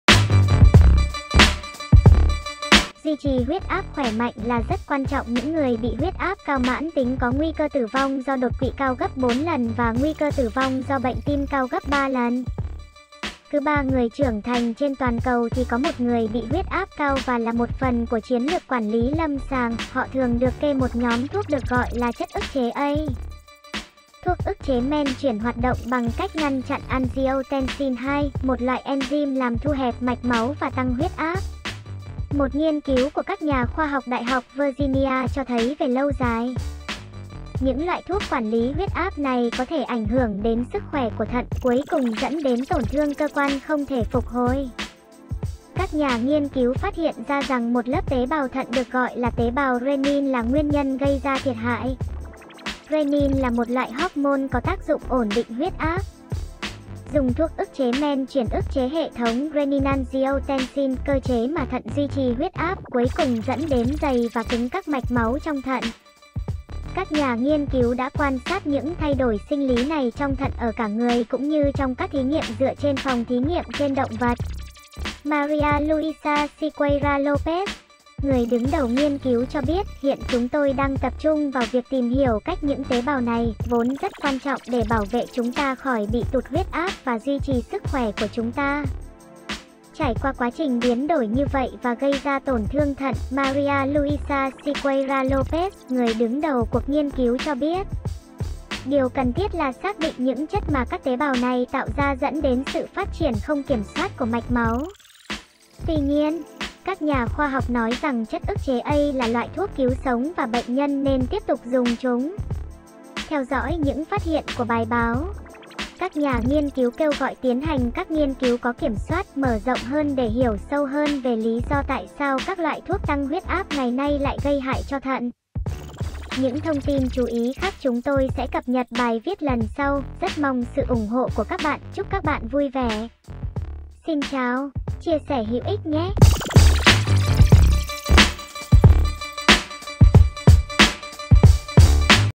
Thuốc huyết áp ảnh hưởng đến sức khỏe thận
Duy trì huyết áp khỏe mạnh là rất quan trọng những người bị huyết áp cao mãn tính có nguy cơ tử vong do đột quỵ cao gấp bốn lần và nguy cơ tử vong do bệnh tim cao gấp ba lần. Cứ ba người trưởng thành trên toàn cầu thì có một người bị huyết áp cao và là một phần của chiến lược quản lý lâm sàng, họ thường được kê một nhóm thuốc được gọi là chất ức chế ACE.

Thuốc ức chế men chuyển hoạt động bằng cách ngăn chặn angiotensin II, một loại enzym làm thu hẹp mạch máu và tăng huyết áp.

Một nghiên cứu của các nhà khoa học Đại học Virginia cho thấy về lâu dài, những loại thuốc quản lý huyết áp này có thể ảnh hưởng đến sức khỏe của thận, cuối cùng dẫn đến tổn thương cơ quan không thể phục hồi.

Các nhà nghiên cứu phát hiện ra rằng một lớp tế bào thận được gọi là tế bào renin là nguyên nhân gây ra thiệt hại. Renin là một loại hormone có tác dụng ổn định huyết áp. Dùng thuốc ức chế men chuyển ức chế hệ thống renin-angiotensin (cơ chế mà thận duy trì huyết áp), cuối cùng dẫn đến dày và cứng các mạch máu trong thận.

Các nhà nghiên cứu đã quan sát những thay đổi sinh lý này trong thận ở cả người cũng như trong các thí nghiệm dựa trên phòng thí nghiệm trên động vật.

Maria Luisa Sequeira Lopez, người đứng đầu nghiên cứu cho biết: “Hiện chúng tôi đang tập trung vào việc tìm hiểu cách những tế bào này, vốn rất quan trọng để bảo vệ chúng ta khỏi bị tụt huyết áp và duy trì sức khỏe của chúng ta, trải qua quá trình biến đổi như vậy và gây ra tổn thương thận” , Maria Luisa Sequeira Lopez , người đứng đầu cuộc nghiên cứu cho biết.

"Điều cần thiết là xác định những chất mà các tế bào này tạo ra dẫn đến sự phát triển không kiểm soát của mạch máu."

Tuy nhiên, các nhà khoa học nói rằng chất ức chế ACE là loại thuốc cứu sống và bệnh nhân nên tiếp tục dùng chúng. Theo dõi những phát hiện của bài báo, các nhà nghiên cứu kêu gọi tiến hành các nghiên cứu có kiểm soát, mở rộng hơn để hiểu sâu hơn về lý do tại sao các loại thuốc tăng huyết áp ngày nay lại gây hại cho thận.